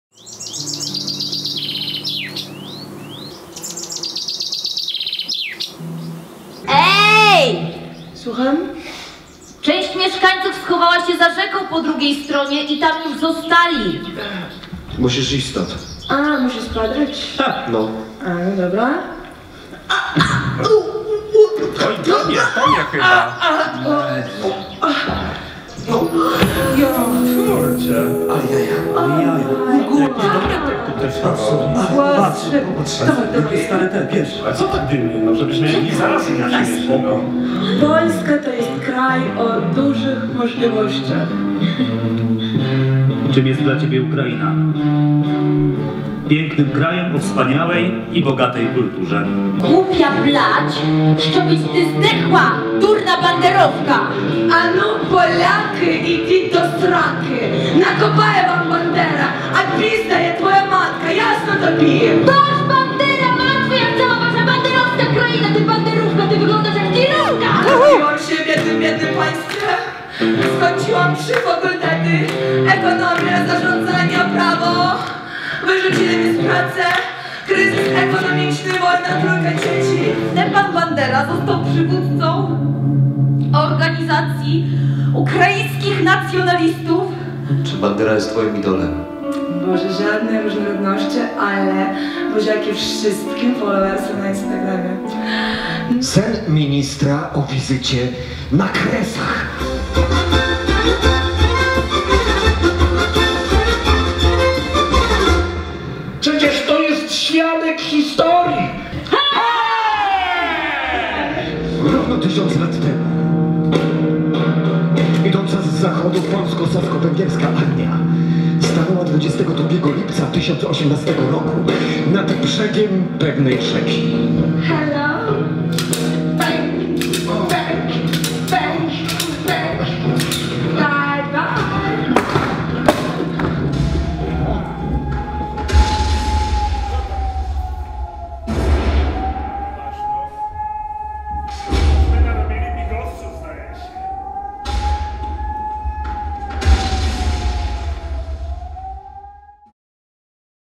"Lwów nie oddamy" reż. Katarzyna Szyngiera - trailer
Premiera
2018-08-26

Obsada
Oksana Czerkaszyna, Dagny Cipora, Małgorzata Machowska, Mateusz Mikoś, Piotr Mieczysław Napieraj, Robert Żurek, Jacek Sotomski (gitara)

Autor
reportażu: Katarzyna Szyngiera, Mirosław Wlekły Video ze Lwowa: Miłosz Kasiura

Reżyseria
Katarzyna Szyngiera

Scenariusz
Katarzyna Szyngiera, Marcin Napiórkowski, Mirosław Wlekły

Scenografia
Przemysław Czepurko, Katarzyna Ożgo

Kostiumy
Ireneusz Zając

Inspicjent i sufler
Małgorzata Depta

Dramaturgia
Olga Maciupa

Światło
Michał Stajniak

Muzyka
Jacek Sotomski

Czas trwania
100 min